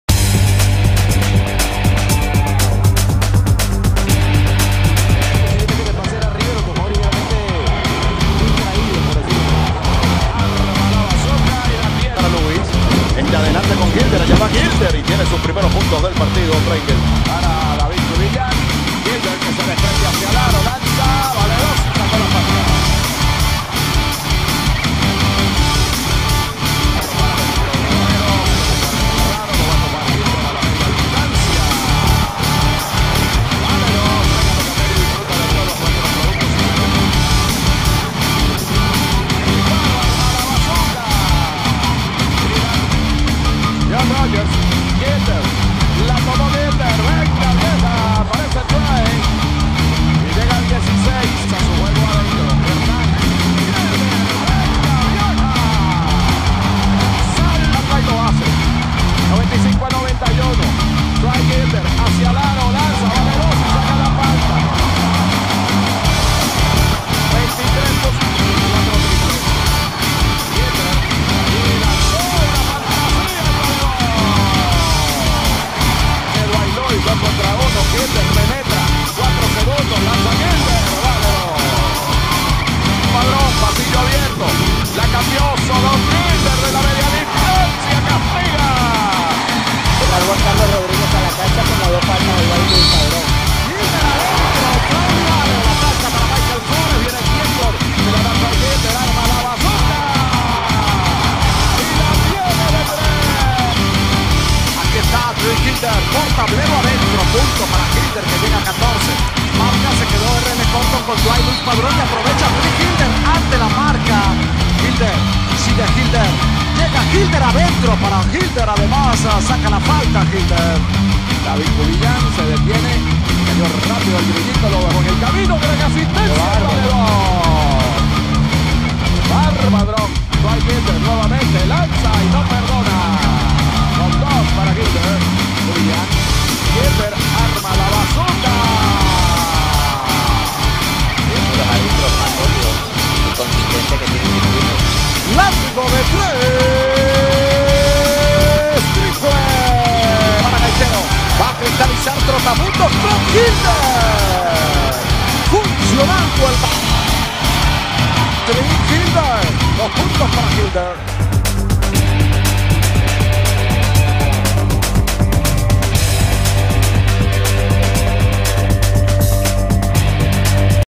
Trey Gilder Highlights ED
Algunos Highlights de la temporada 2013 de Trey Gilder en nuestra LPB